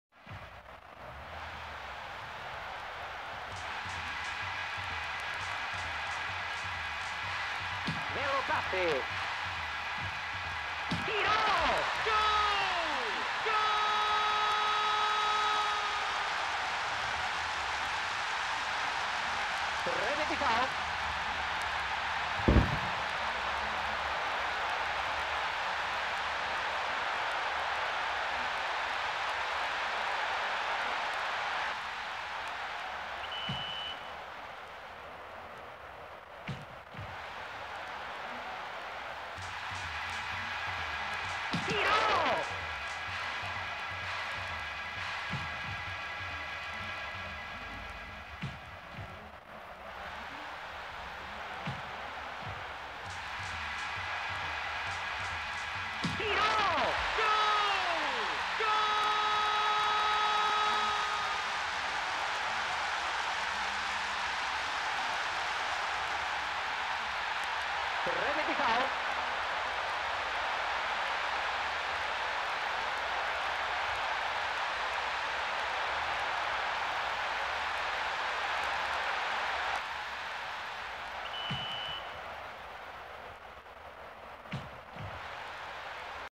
[FiC + SD] TV Tupi | Trecho do Jogo: Copa da França 98 - 14/10/2020
Isso Foi Gravado às 4h34 da Manhã durante a Exibição de Jogos da Madrugada Brasil X Peru.

Aviso:
Este Canal é Versão Fictícia! Não Queremos Infrigir Críticas Sobre a Obra de Montagens em Ficção.